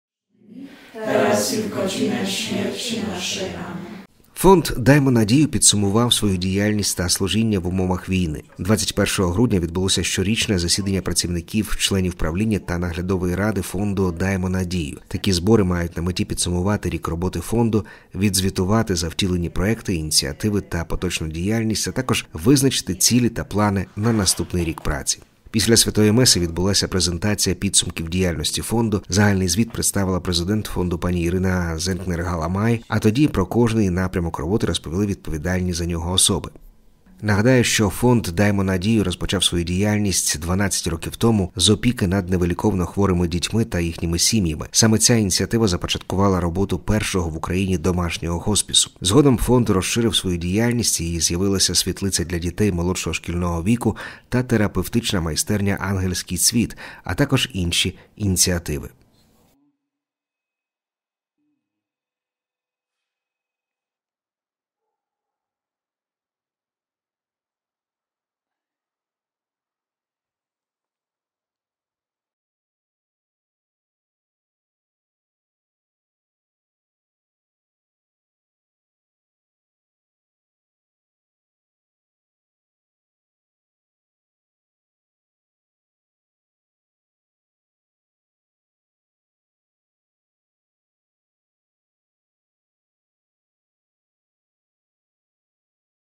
Фонд «Даймо Надію» підсумував свою діяльність та служіння в умовах війни.
21 грудня відбулося щорічне засідання працівників, членів правління та наглядової ради Фонду «Даймо Надію». Такі збори мають на меті підсумувати рік роботи Фонду: відзвітувати за втілені проєкти, ініціативи та поточну діяльність, а також визначити цілі та плани на наступний рік праці.

Після Святої Меси відбулася презентація підсумків діяльності Фонду. Загальний звіт представила президент Фонду п. Ірина Зенкнер-Галамай, а тоді про кожний напрямок роботи розповіли відповідальні за нього особи.

Сестра Лідія Зембович FRM, директорка парафіяльної світлиці, розповіла про про діяльність парафіяльної світлиці, яка займається дітьми. Іванна Андрейчук, віце-президент Фонду, говорила про численні плоди служіння та підкреслила особливу роль підопічних дітей хворих на СМА, які вже відійшли до Дому Отця: "Ми маємо багатьох ангелів в небі, завдяки яким наш фонд може служити та творити добро". Отже, не зупинився медичний супровід підопічних домашнього госпісу, про що на зібранні поінформувала п. Іванна Андрейчук, яка протягом останнього року війни продовжувала здійснювати виїзди до пацієнтів у порядку, усталеному ще до війни. Тож усі підопічні сім’ї мають належний догляд та забезпечені усім необхідним завдяки жертводавцям та підтримці іноземних партнерів.

З вересня відновила свою роботу також майстерня «Ангельський цвіт», яка під крилом Фонду опікується дітьми та молоддю з інтелектуальною неповносправністю.

Нагадаємо, що Фонд «Даймо Надію» розпочав свою діяльність 12 років тому з опіки над невиліковно хворими дітьми та їхніми сім’ями. Саме ця ініціатива започаткувала роботу першого в Україні домашнього госпісу. Згодом Фонд розширив свою діяльність і з’явилася світлиця для дітей молодшого шкільного віку та терапевтична майстерня «Ангельський цвіт», а також інші ініціативи.